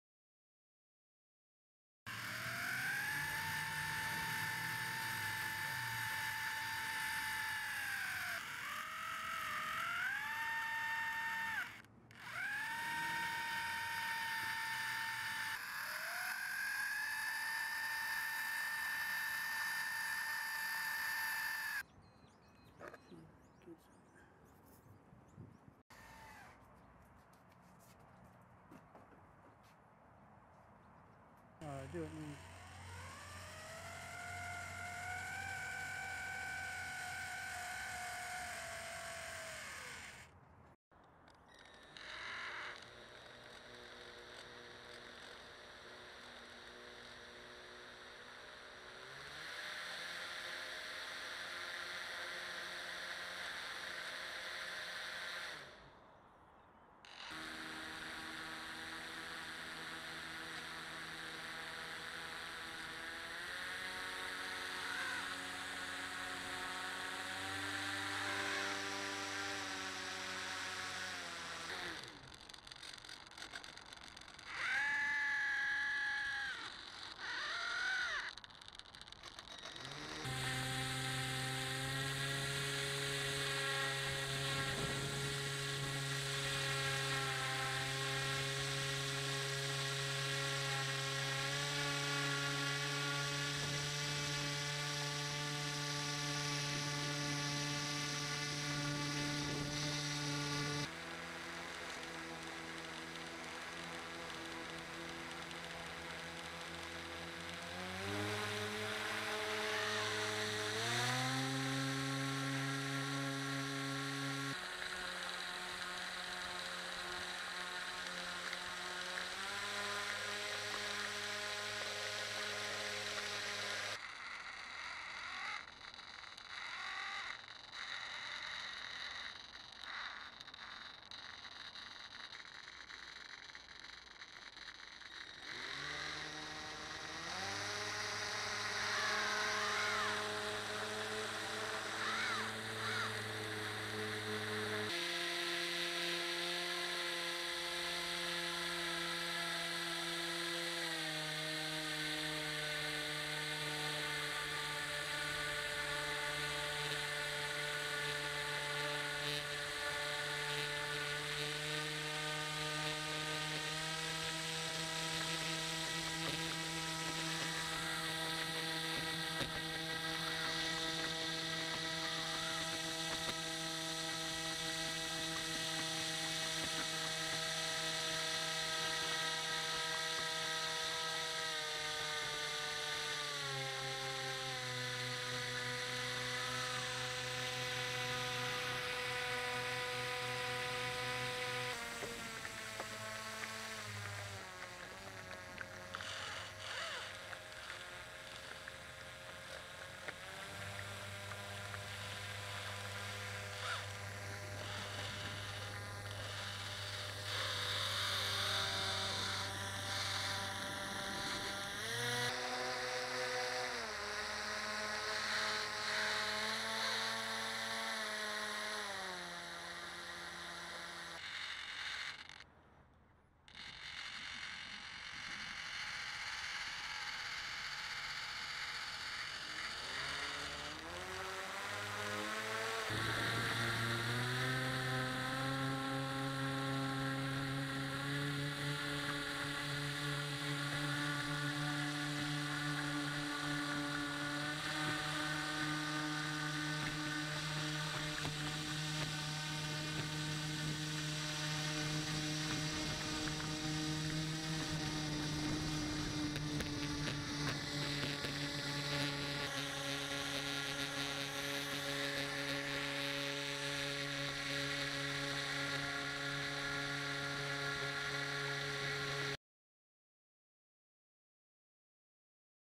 Garys Camera Pan Mount
Springfield Lakes Maritime Modellers.
Mobius Camera mounted on rescue boat with pan mount
Videoing some Dragons on the Lake